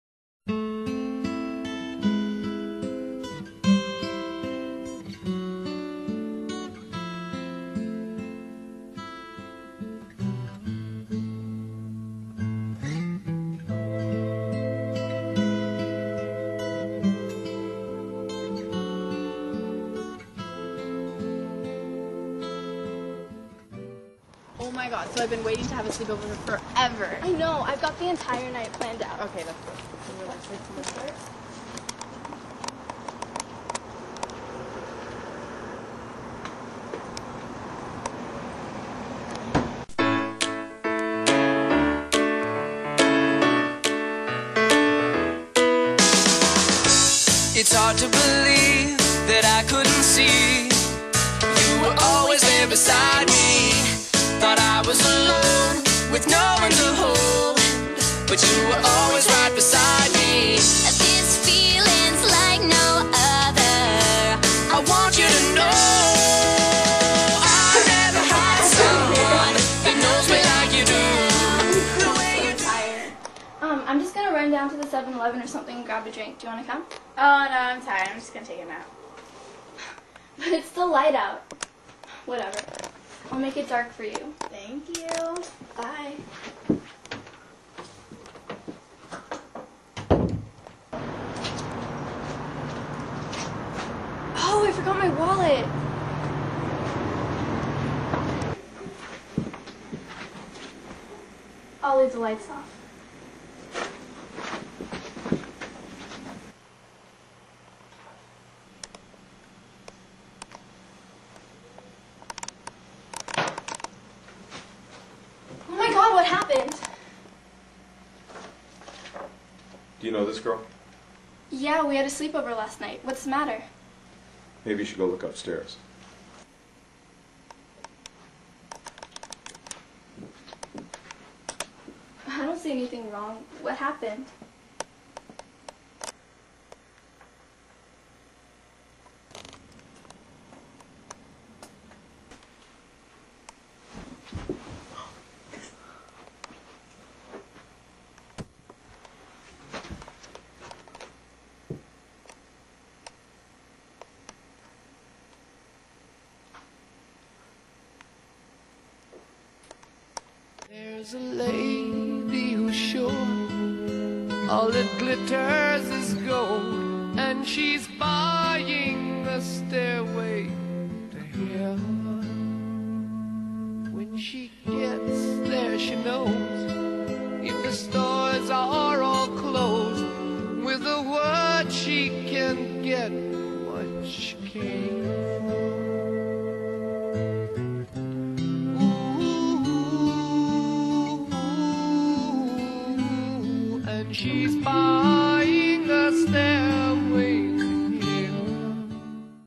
Afraid of the Dark Final Version
Two young girls have a sleepover but something sinister happens. Based on the real life events from the BTK killer.